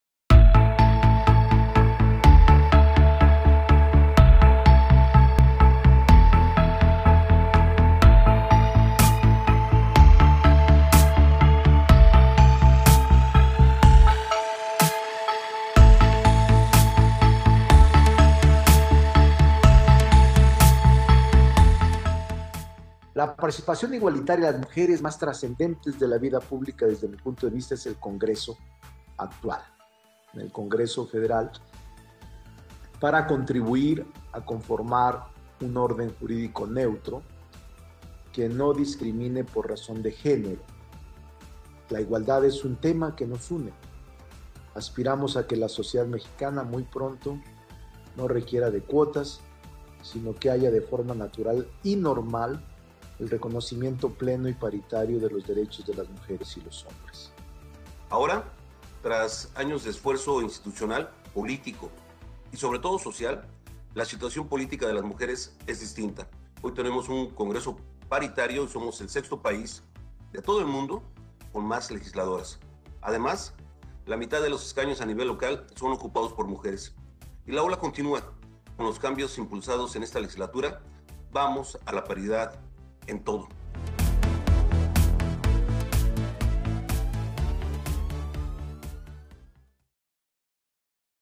Presentación del Libro "Del Sufragio a la Paridad. Evolución de la participación electoral de..."
Presentación del Libro "Del Sufragio a la Paridad. Evolución de la participación electoral de las Mujeres en México" / Senado de la República